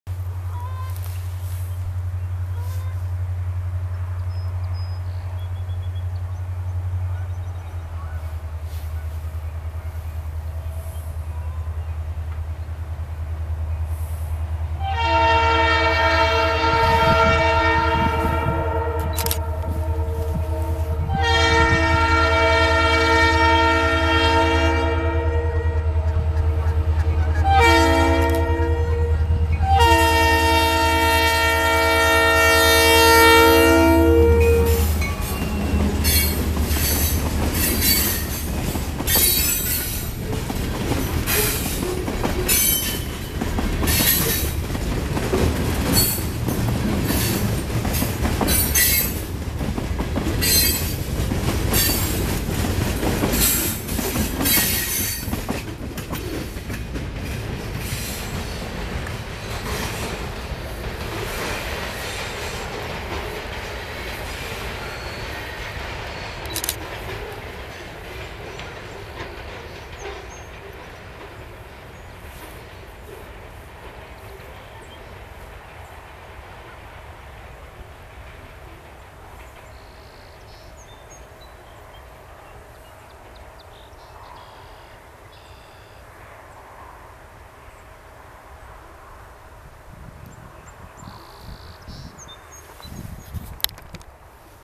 ITHACA CENTRAL RR ITHR101 ENGINE4241 GOING LONG HOOD FORWARD TOWNLINE CROSSING IN VAN ETTEN NY
ITHACA CENTRAL RR ITHR101 ENGINE4241 GOING LONG HOOD FORWARD TOWNLINE CROSSING IN VAN ETTEN NY HEADING TO SAYRE YARD